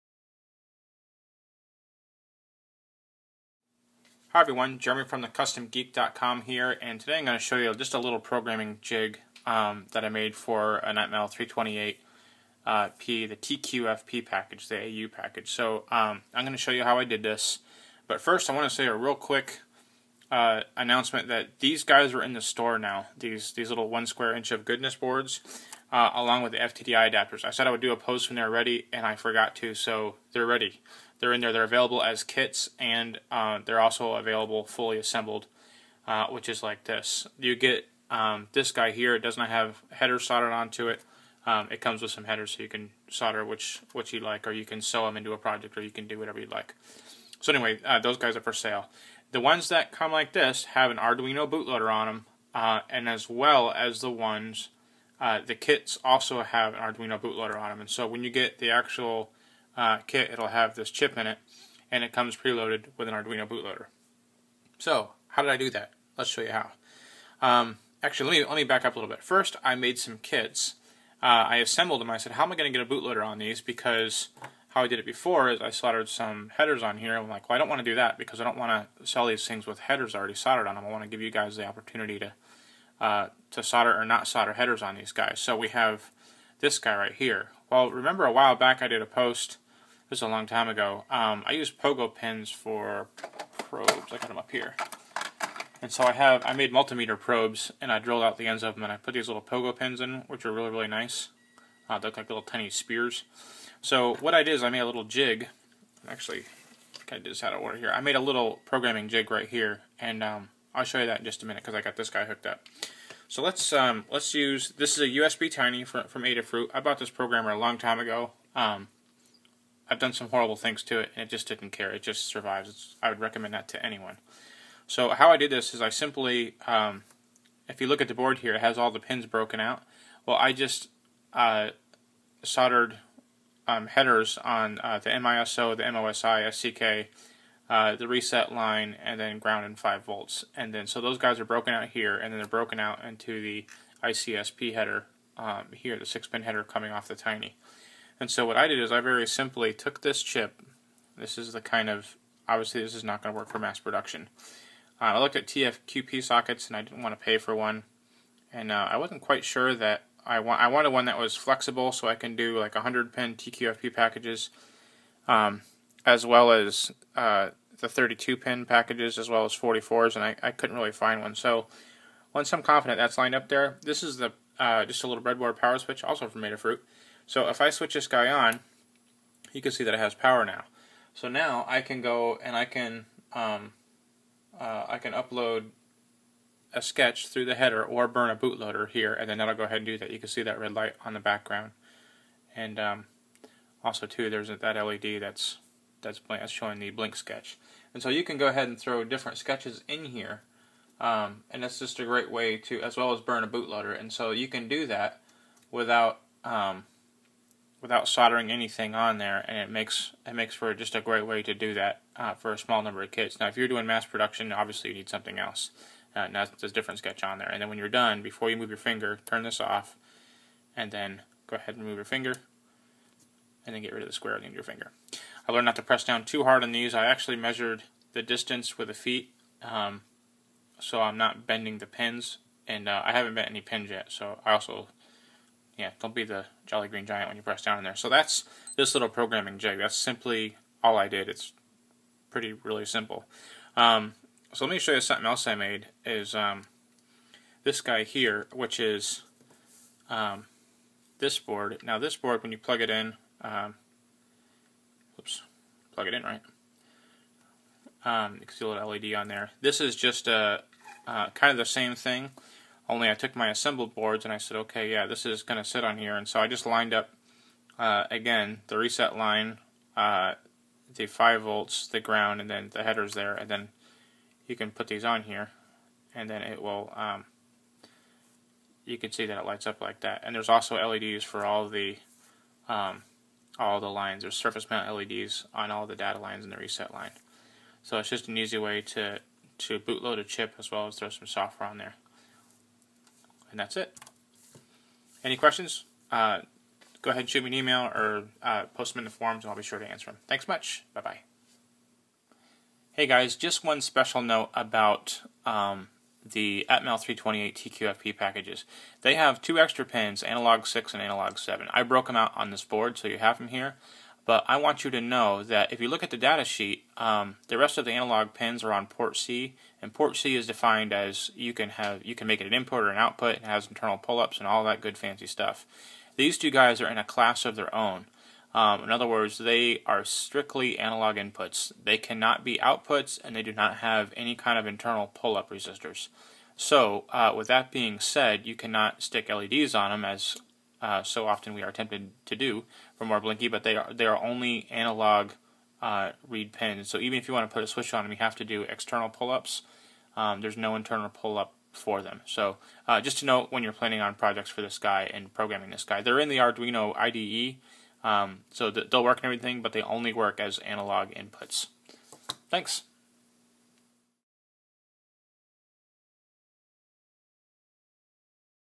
Programming the Square Inch - TQFP Jig
First, I am happy to announce that the One Square Inch of Goodness and the FTDI Adapter are in the stock now and available for purchase!

In the video above, I show how I programmed the assembled kits as well as the raw TQFP IC's without soldering anything to them. It's a pretty simple setup, but solved the problem I was facing as well as made things a lot faster. As I mentioned in the video, pogo pins are found here, USBtinyISP here, and breadboard switch here (from Adafruit).

A note on using ICSP: - The Square Inch is designed to accept an FTDI connection for programming. Because of this, a .1µF capacitor is between the reset line of the ATmega328P and the pin labeled DTR. This is so when you load new sketches on the Square Inch, it will reset automatically via the DTR line on the FTDI adapter. When you use ICSP, you need to bypass this capacitor! Otherwise it will not work. The programming board I used in the video had the capacitor bypassed with solder (pictured below). There is also a via to the right of the capacitor that leads to the reset line. This can be shorted to the DTR pin to accomplish the same task, as I did to program the assembled boards.

On the assembled boards, if you wish to change the bootloader or upload sketches via ICSP, you can temporarily short the cap using tweezers on the cap itself (green arrows) or the DTR pin and the via leading to the reset line of the 328 (red arrows) as shown in the picture below.